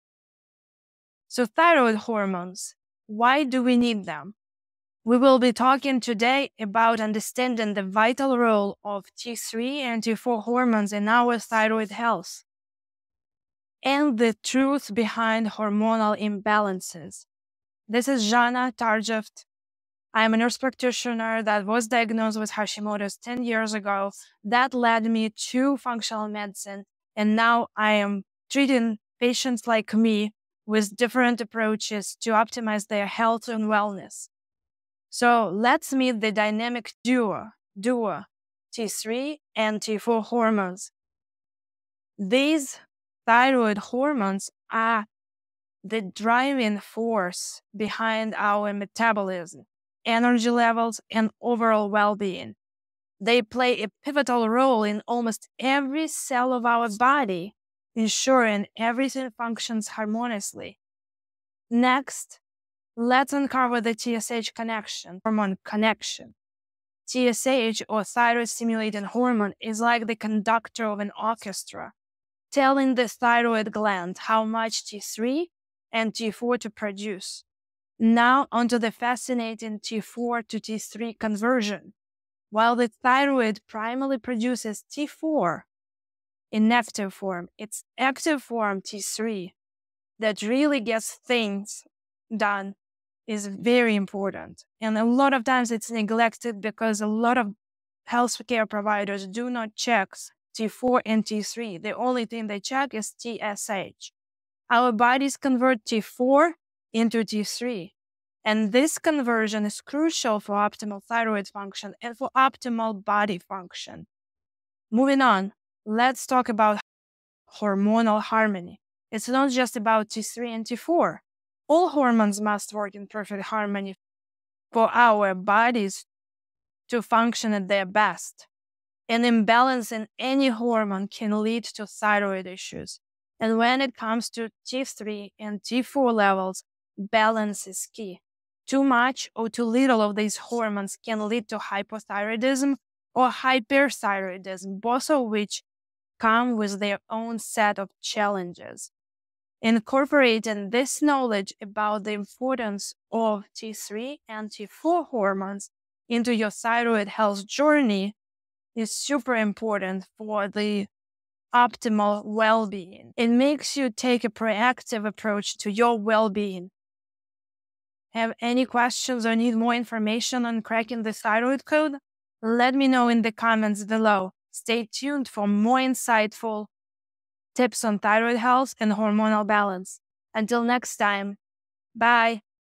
Thyroid Hormones The Essential Key to Your Energy and Wellness!
I thought that Hashimoto's and hypothyroidism happen rarely - while I was learning about the diseases of the thyroid gland on my way to becoming a medical professional. When I got diagnosed with Hashimoto's 10 years ago, there was little information about it and how to treat it. I also suffered from environmental allergies (my skin test showed allergies to every tree type in Arizona) and gut issues, just to name a few issues at that time. I slowly put the puzzle together of my root causes.

I am sharing my journey and knowledge when it comes to dealing with this chronic autoimmune disease.